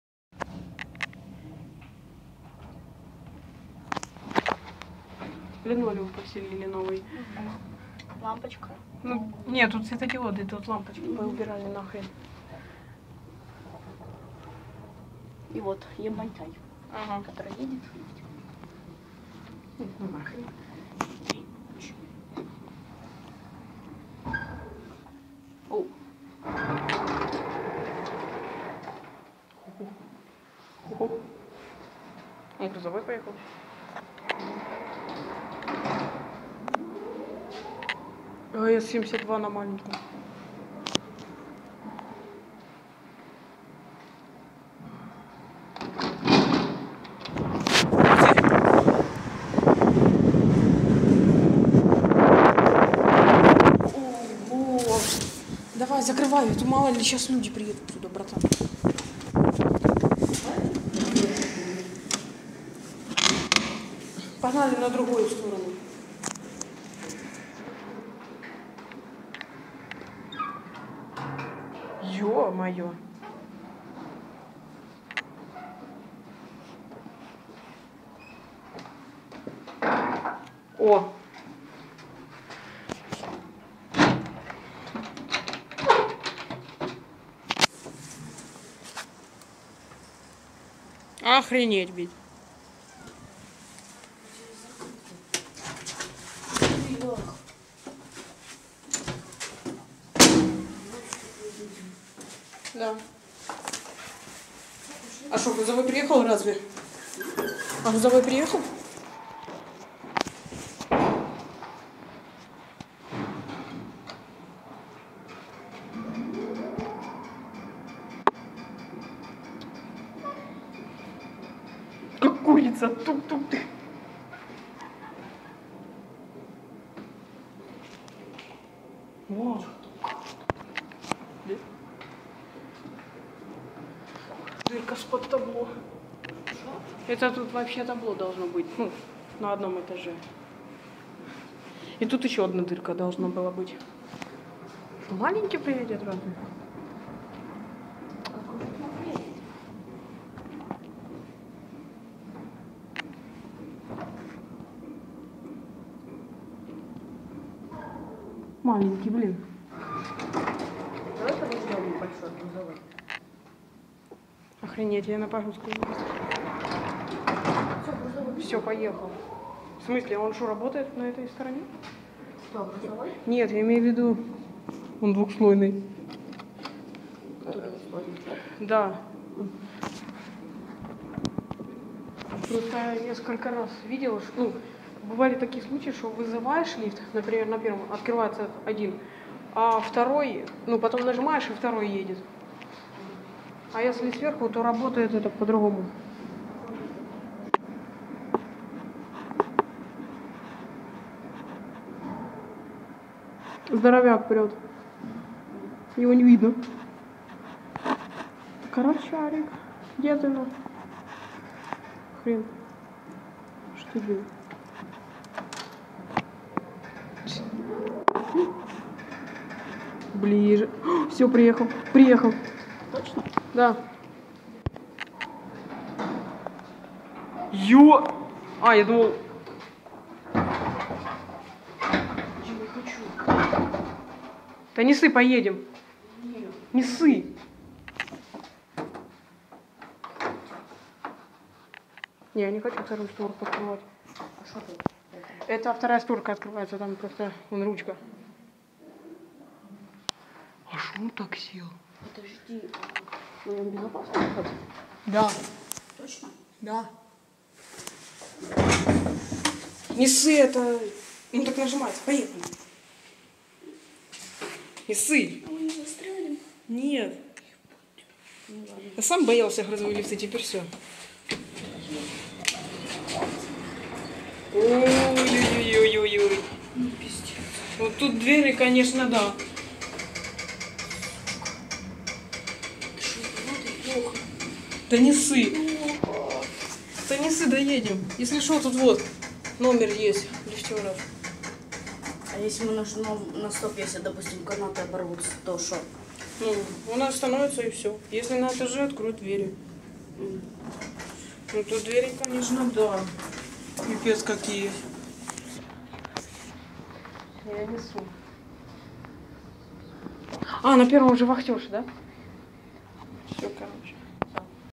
(г. Харьков) Лифты МЛЗ и КМЗ ≈1988 г/п 320 и 500 кг скорость 1.0 м/с(156)
Пассажирские

м. Харків, ж/м Роганський, вул. Зубарєва 37

г. Харьков, Ж/М Роганский, Ул. Зубарева 37